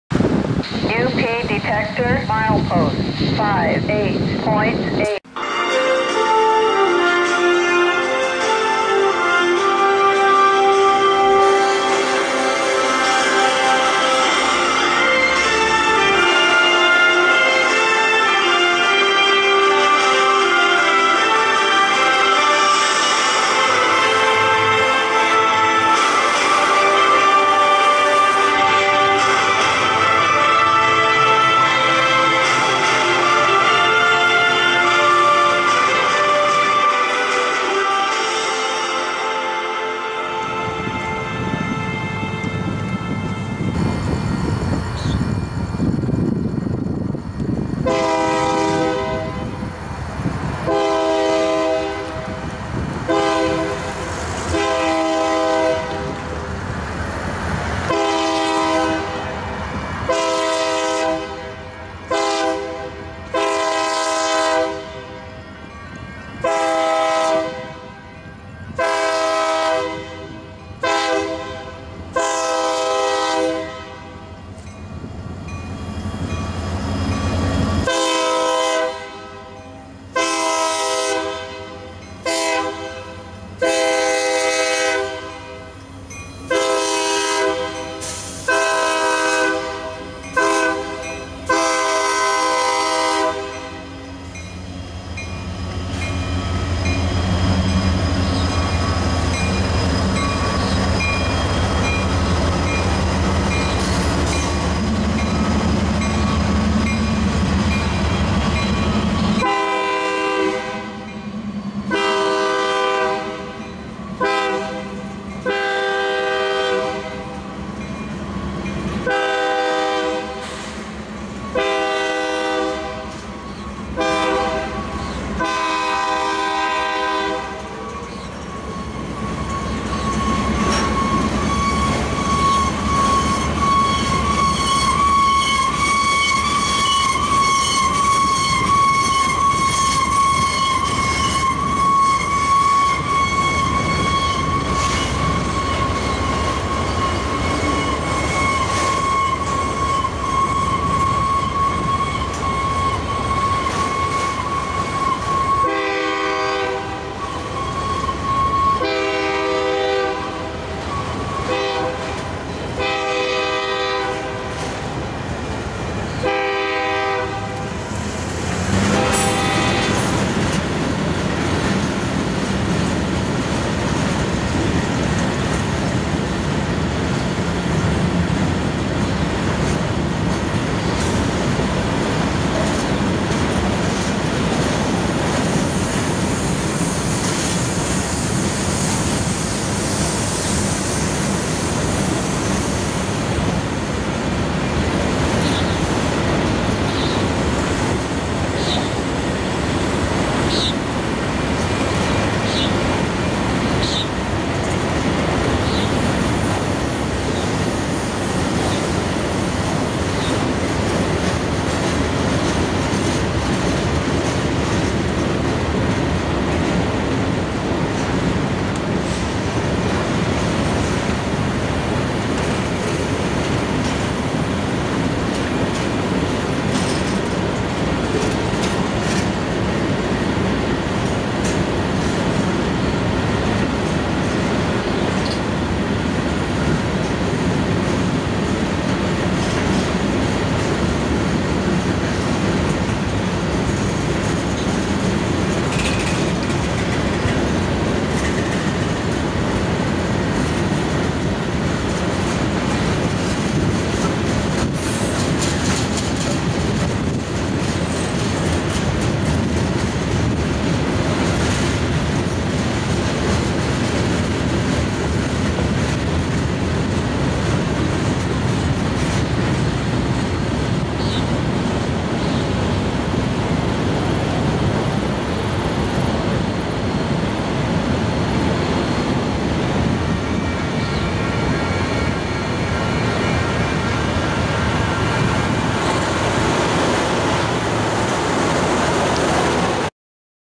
UP #4185 Leads South Bound Manifest Freight Train
A south bound manifest detoured on to the Cuero Subdivision at the Esplanade Street crossing in Cuero, Texas.